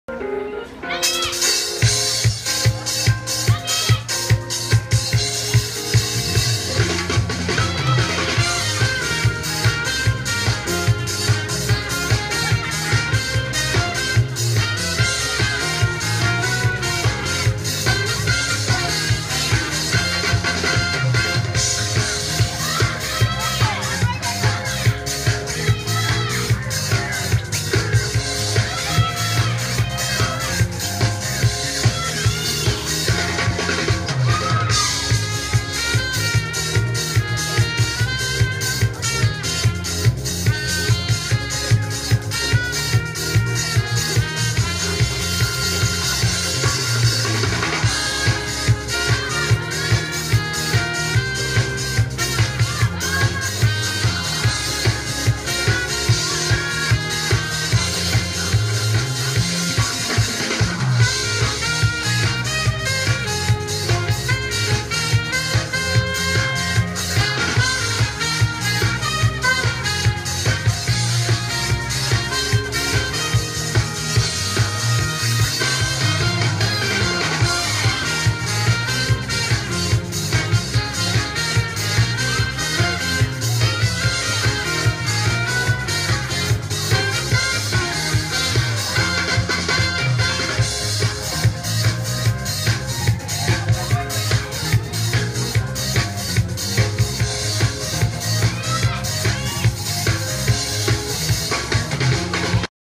D'z mix ⑤ 2019/7/15 アリオ倉敷
2019年7月15日（月・祝）16:00～
アリオ倉敷屋外ステージ
「SAKE SPRING 倉敷」MUSIC LIVE